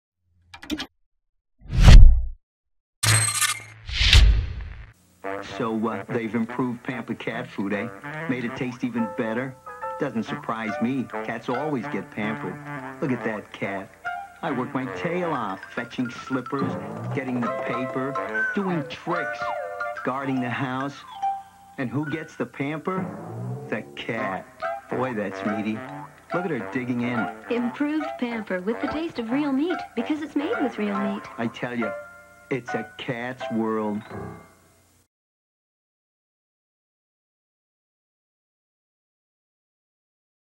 Funny Pamper Cat Food Commercial 1984 Cat & Talking Basset Hound
Funny television commercial from 1984 for Pamper cat food with a long haired cat and a talking basset hound.